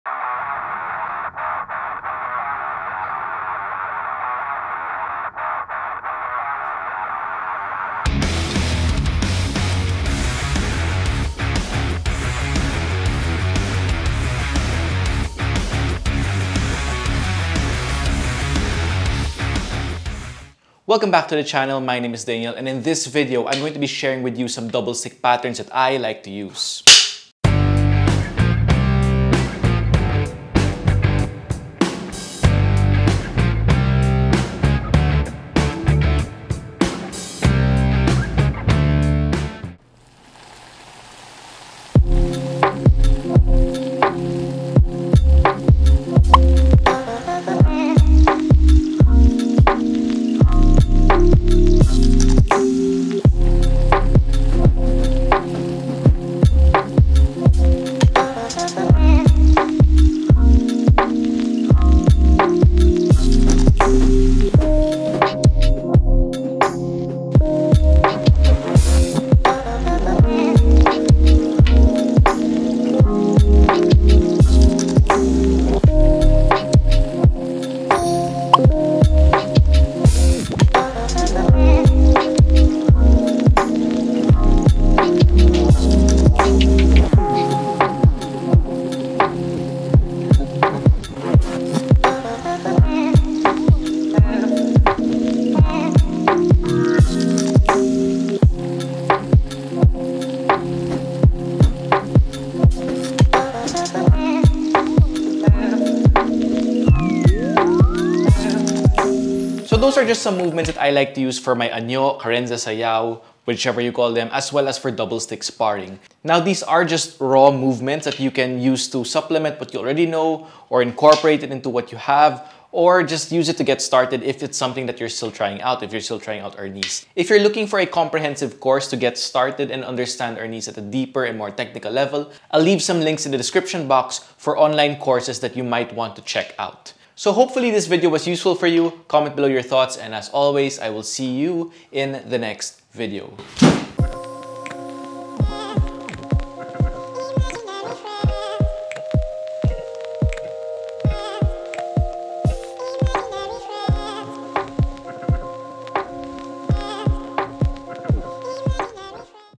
Double Stick Patterns | Arnis Kali Eskrima
Sharing some double stick patterns I like to use for my Arnis anyo and sparring practice.

Leave a comment to let me know your thoughts, questions, and suggestions for future content!